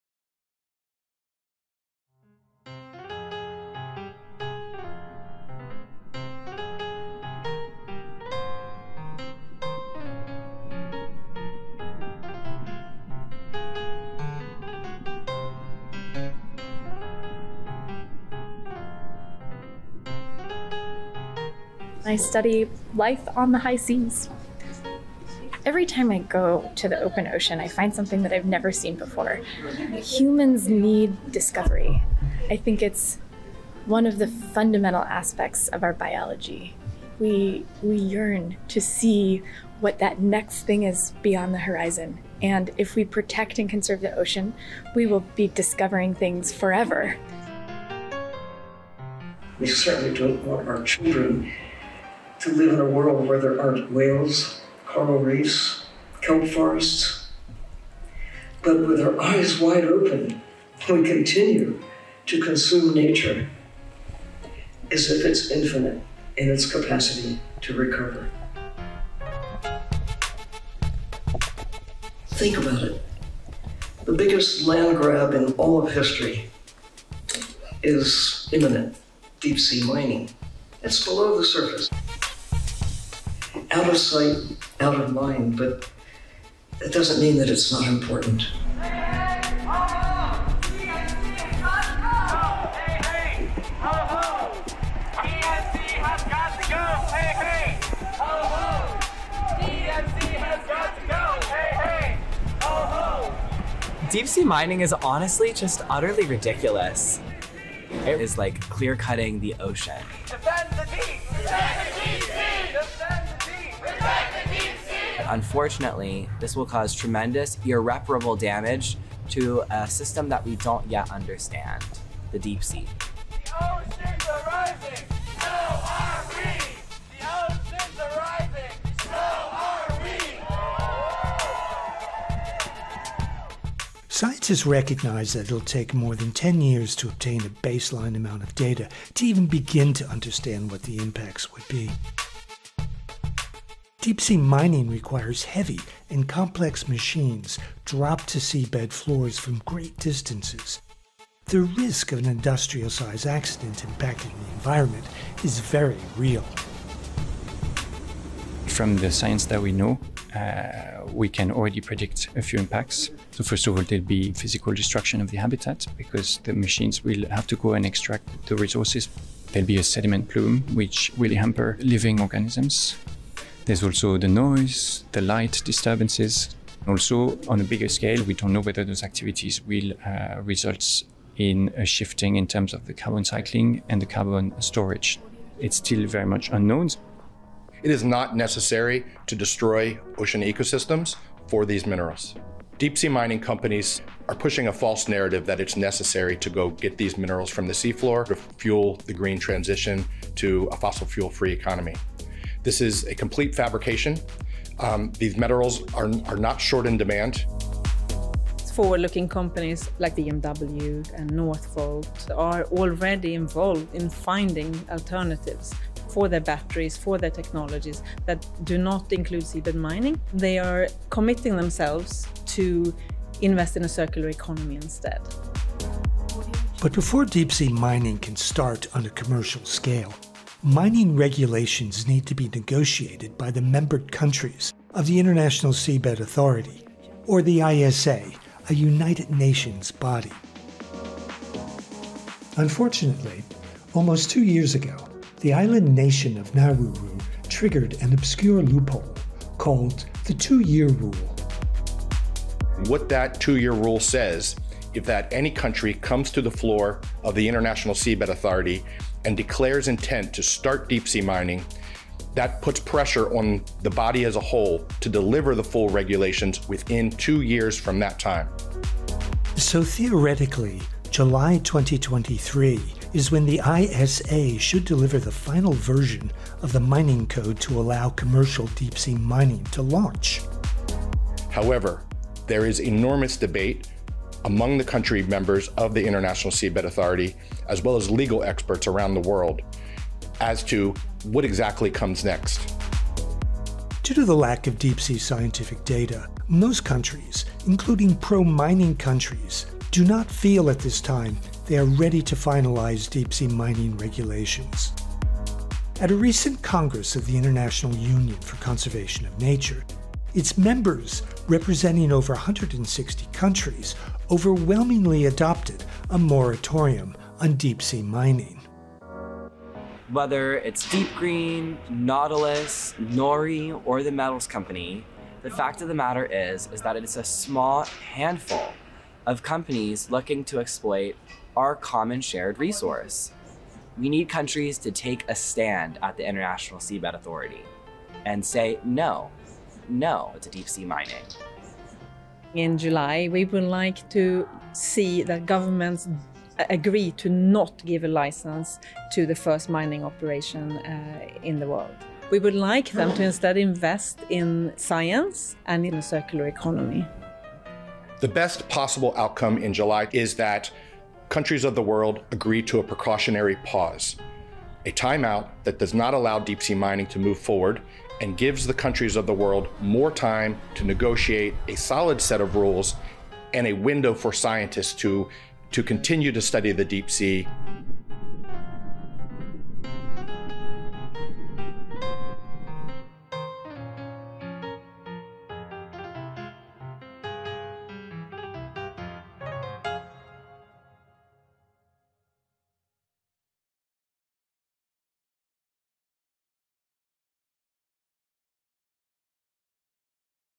The trouble with mining the deep
The IUCN union has called for a moratorium on Deep-sea and Seabed Mining. This is why.